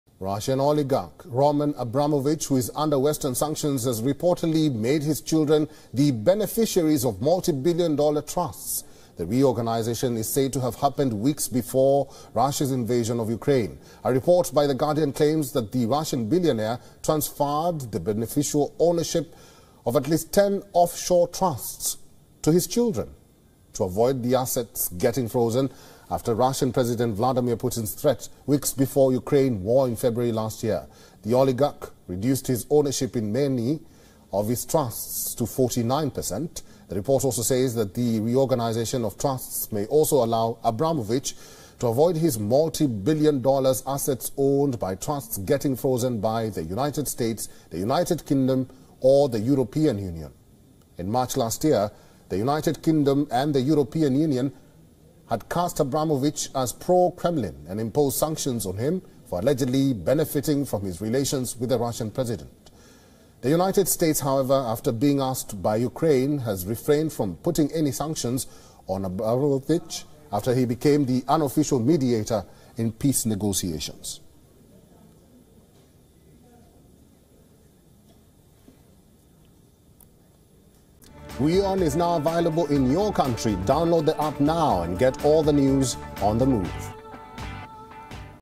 Russia: Billionaire Abramovich transfers trust ownership to skip sanctions | Latest News | WION
Russian oligarch Roman Abramovich who is under Western sanctions has reportedly made his children the beneficiaries of multi-billion dollar trusts. The reorganization is said to have happened weeks before Russia's invasion of Ukraine.